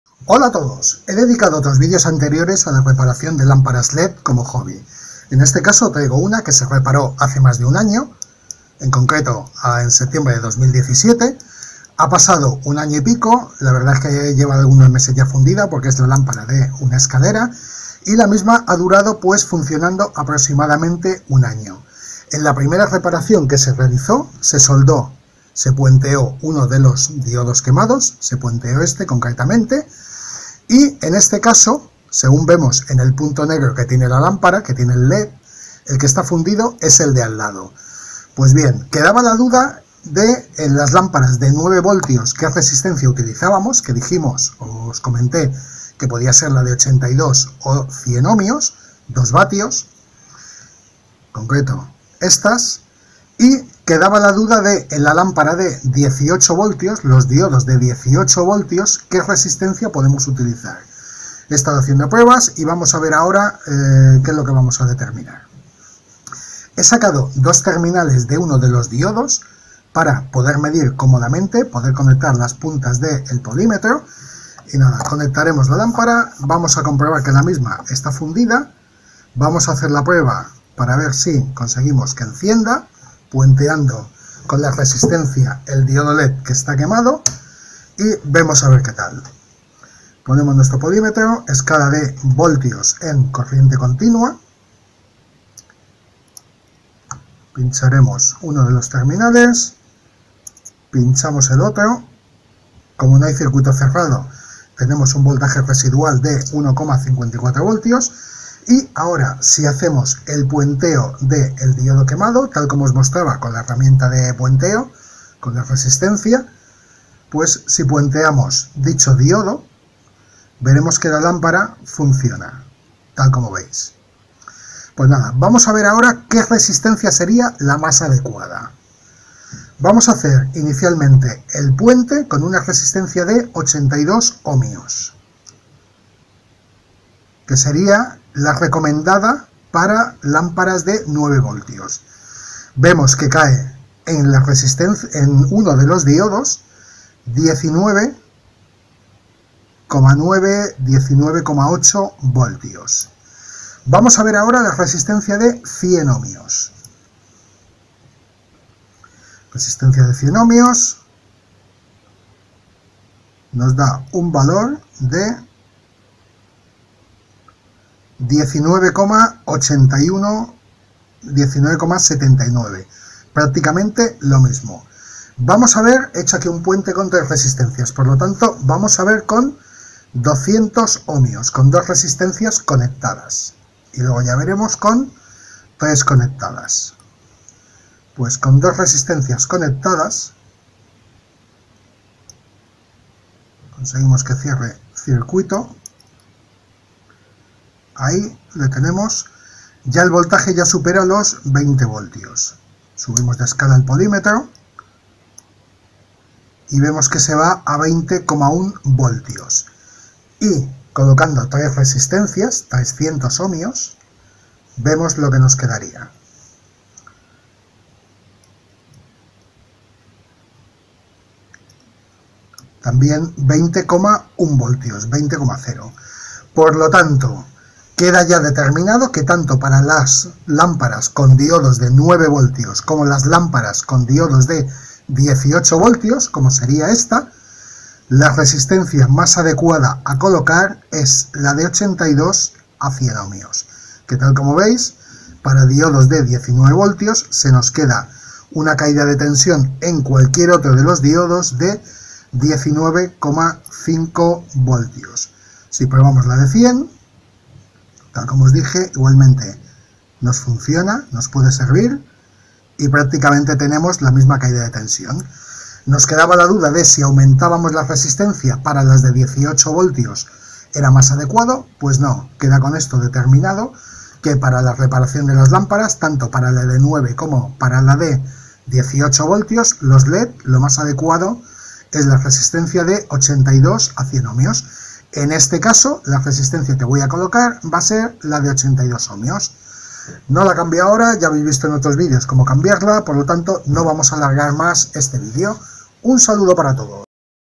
¿QUE RESISTENCIA USO PARA REPARAR UNA LAMPARA LED AVERIADA? 82 OHMS /2W
Veamos que valor de resistencia es el mas adecuado para la reparacion de lampara led, aproximadamente  el valor  de R estaría entre 100 y 200 ohms, si lo calculáramos  pero haciendo pruebas entre varias:  82, 100, 200, 300 ohms veremos como la de 82 OHMS 2 W  es la que mejores resultados de CAÍDA DE TENSIÓN EN EL DIODO presenta, (en este caso se ha probado con leds de 18 V).